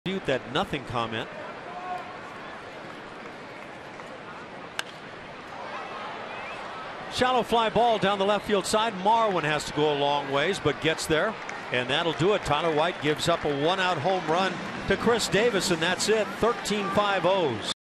HOU@BAL: First baseman White pitches in the 8th
Tyler White makes the first pitching appearance of his career and gets all three outs in the bottom of the 8th inning